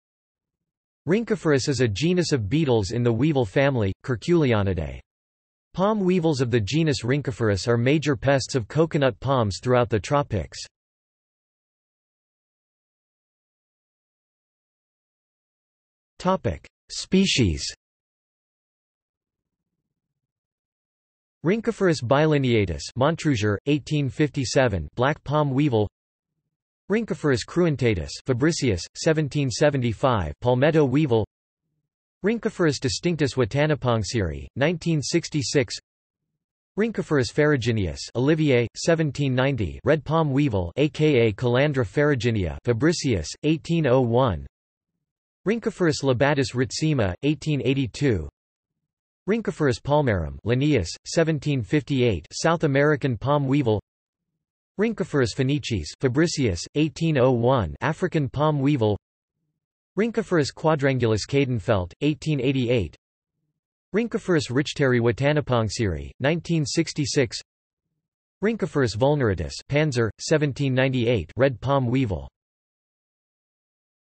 Rhynchophorus | Wikipedia audio article
This is an audio version of the Wikipedia Article:
Rhynchophorus

00:00:12 undefined

- Socrates

SUMMARY
Rhynchophorus is a genus of beetles in the weevil family, Curculionidae. Palm weevils of the genus Rhynchophorus are major pests of coconut palms throughout the tropics.